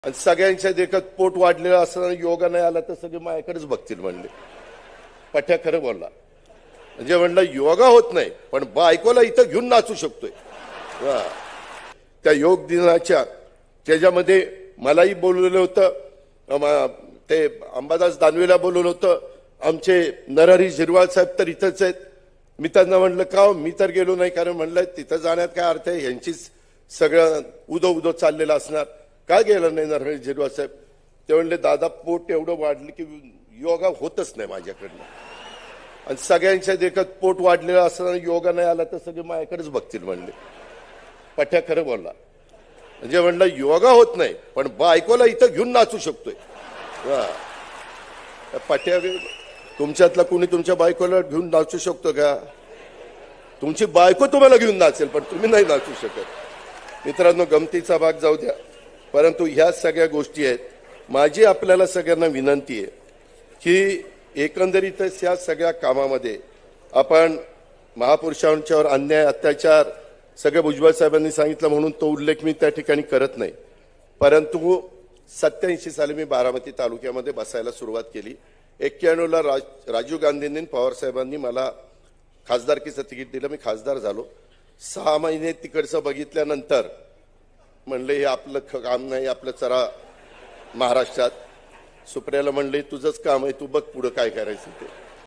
Narhari Zirwal बायकोला खांद्यावर घेऊन नाचले, Ajit Pawar यांनी घेतली फिरकी | Maharashtra Times
राष्ट्रवादी काँग्रेसने बुधवारी (२१ जून) आपलं रौप्य महोत्सवी वर्ष साजरं केलं आहे. यावेळी, पक्षाच्या सर्व बड्या नेत्यांची भाषण झाली, तसेच अजितदादांच्या भाषणाने नेहमीप्रमाणे कार्यक्रमात चांगलीच रंगत आणली आहे. आंतरराष्ट्रीय योग दिनाच्या निमित्ताने बोलताना अजितदादांनी नरहरी झिरवळांची चांगलीच फिरकी घेतली. बायकोला खांद्यावर घेऊन नाचेल, पण योग नाही होणार, वाह रं पठ्ठ्या; असं दादा झिरवळांना उद्देशून म्हणाले.